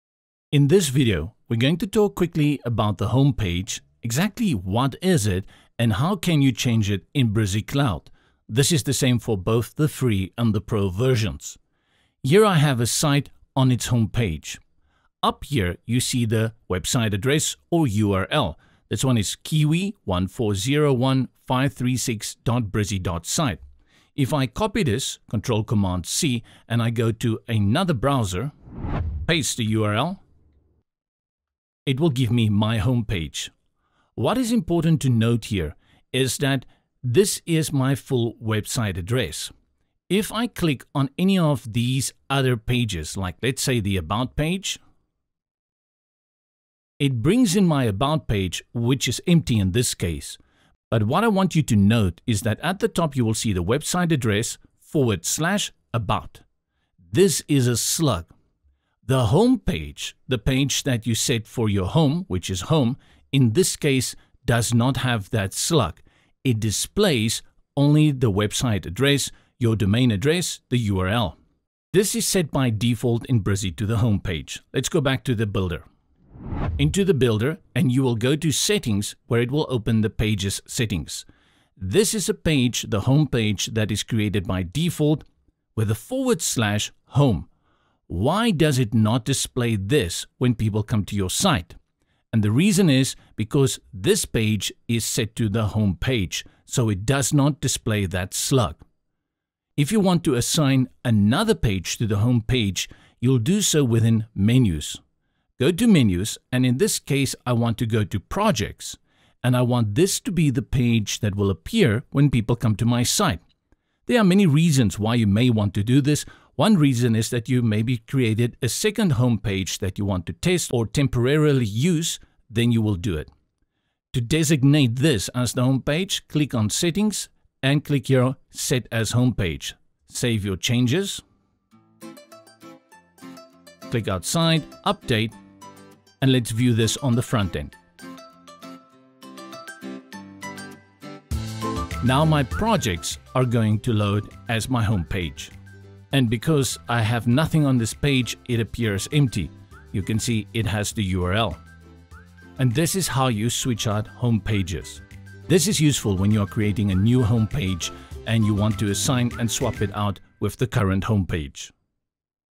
Brizy Tutorial: Designate Your Homepage in Minutes
👋 Brizy assigns the first page created by default as your homepage. You can easily designate this function to any other page you've created.

Brizy Cloud FREE means FREE: no costs ever. Free builder, free external popups, and free subdomains. Unlimited websites and unlimited external popups. Get crackin'!